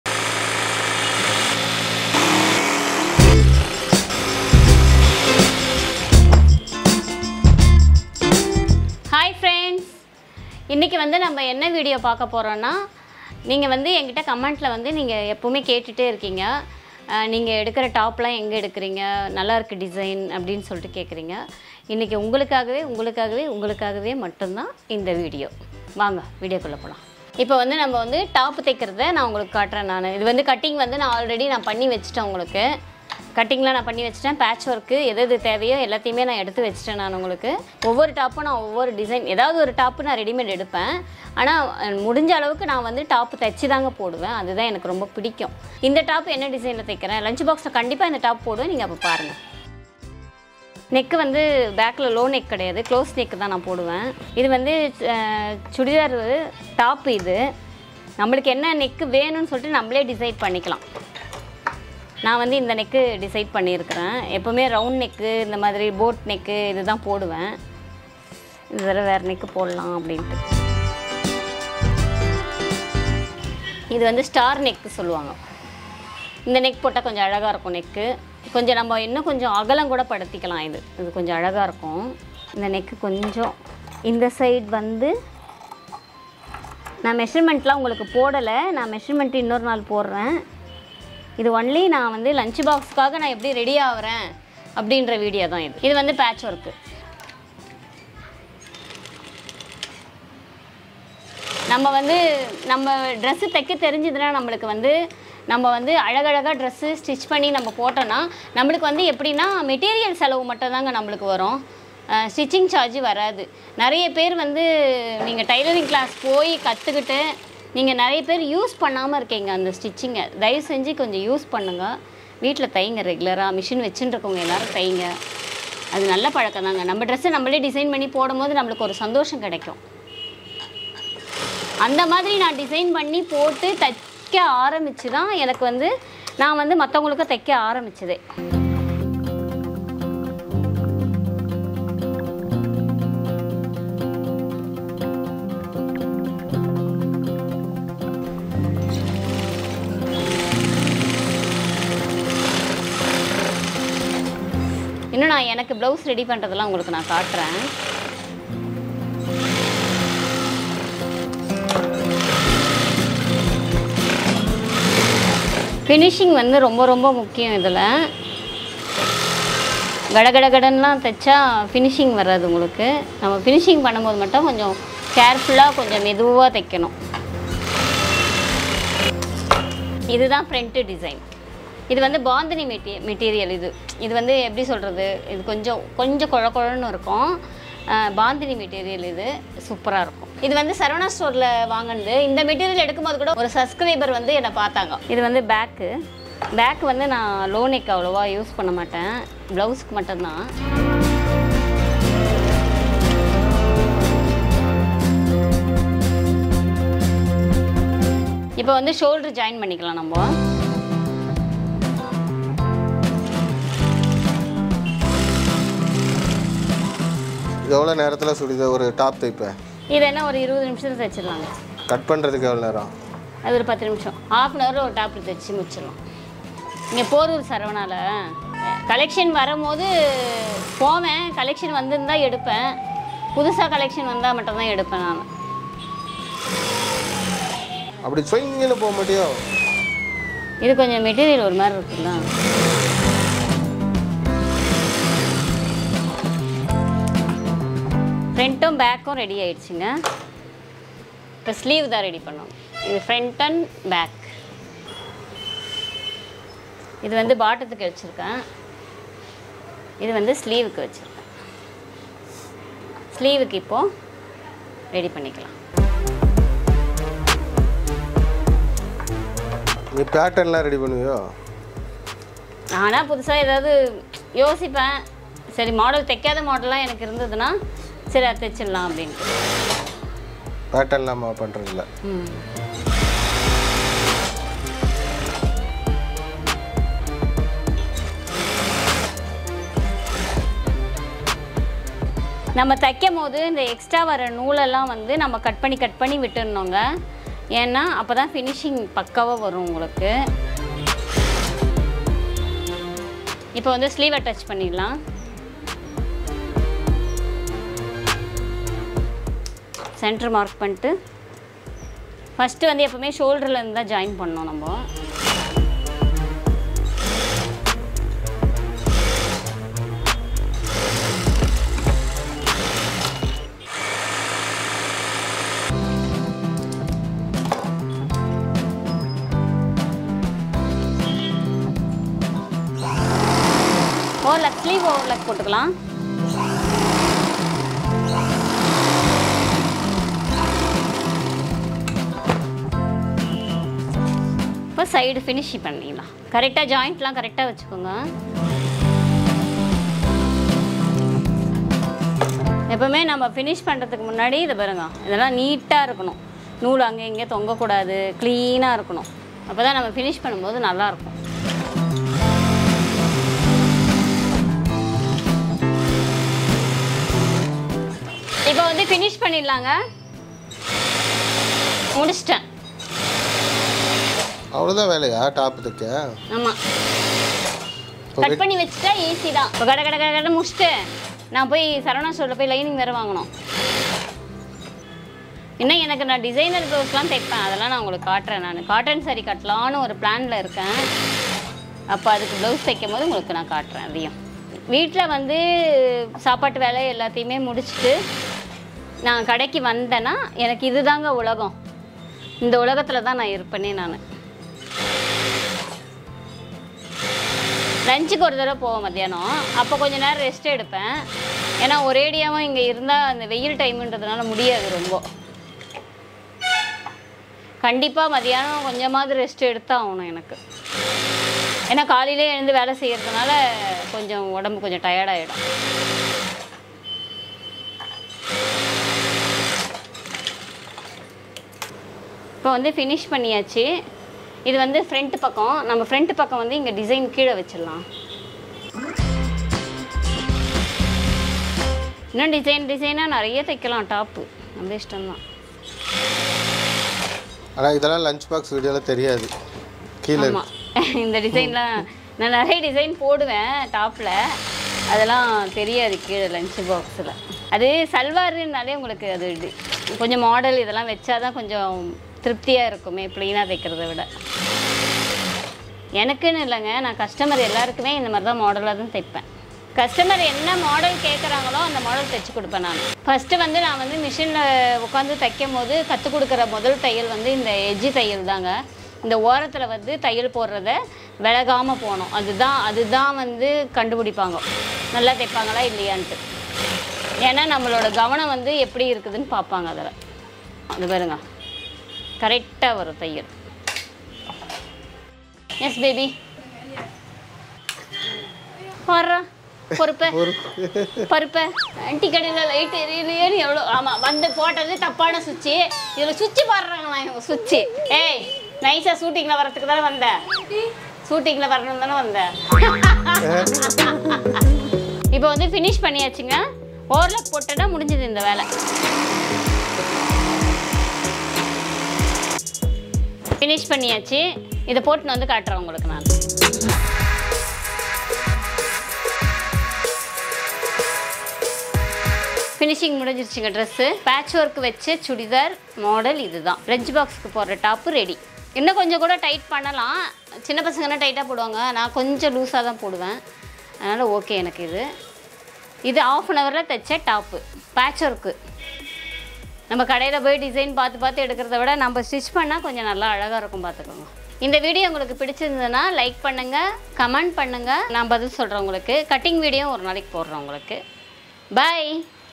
Lunch Box Costumes : Designer Chudidhar Stitching 👗🥻| பேட்ச் ஒர்க் சுடிதார் | Jeni
Lunch box - க்கு டாப் தெச்சாச்சு ...